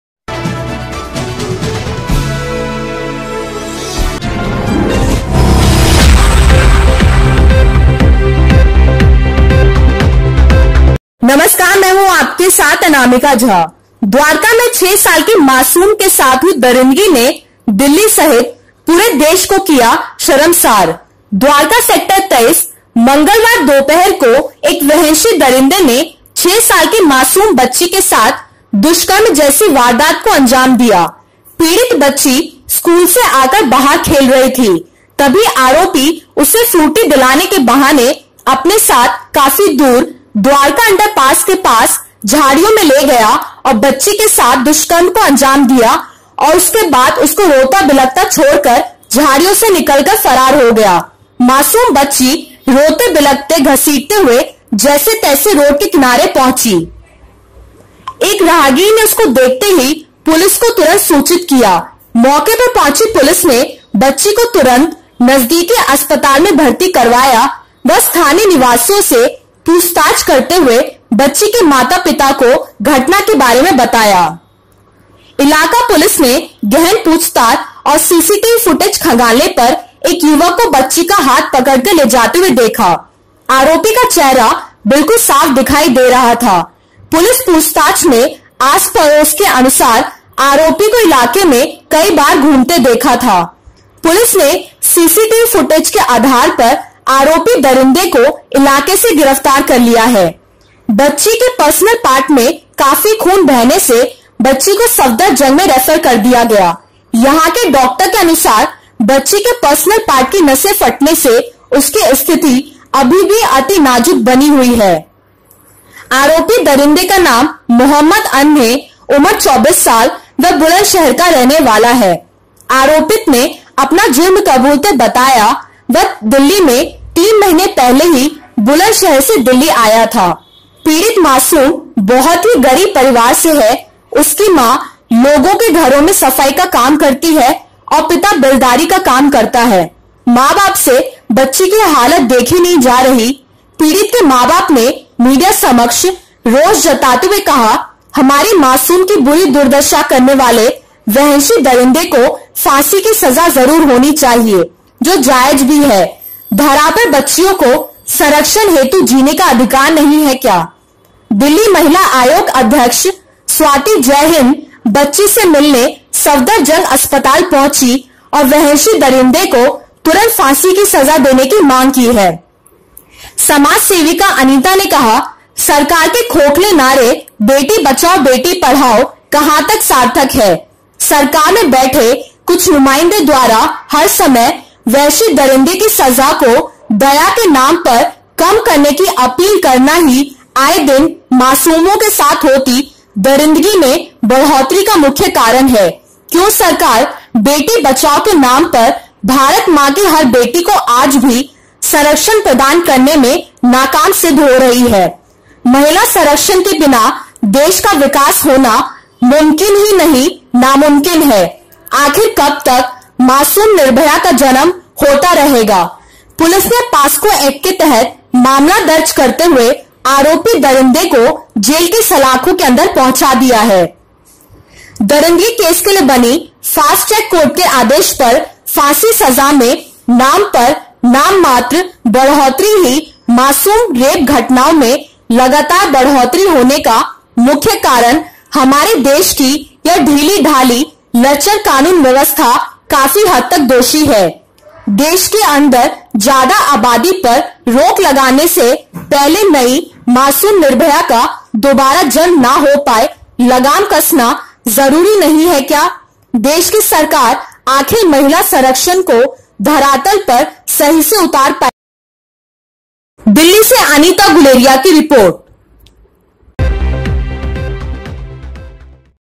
Delhi :छ: साल की बच्ची के साथ दरिंदगी करने वाला गिरफ्तार
Report - Anita Guleria.